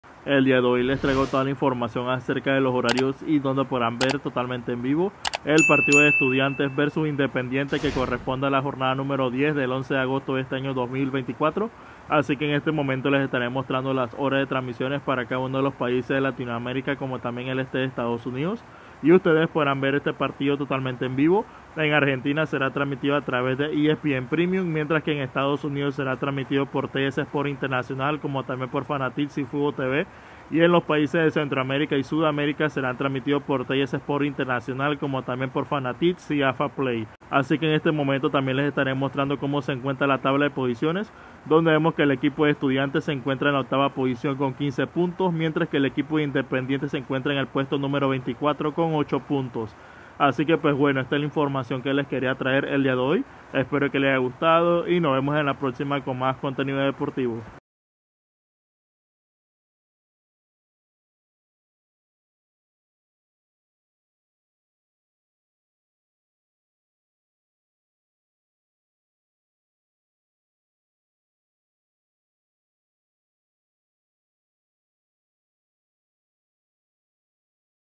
⚽️Estudiantes vs Independiente⚽️Dónde Ver El Partido EN VIVO|Jornada 10 Liga de Argentina 2024
En este video les estaré mostrando todo acerca de los horarios como tambien donde podrán ver el partido de Estudiantes vs Independiente en la Jornada 10 de la Liga Profesional de Futbol Argentina 2024.